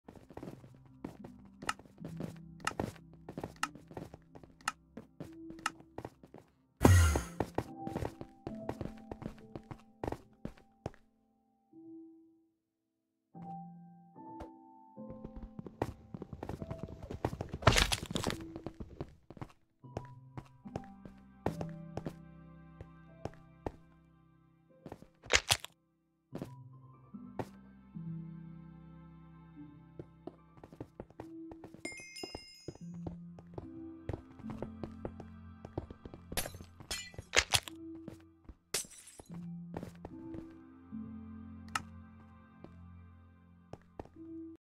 Lifeboat Hacker (VIP) Badboyhalo34202
Really sad to see paying customers run around cheating and hacking.  disgusting.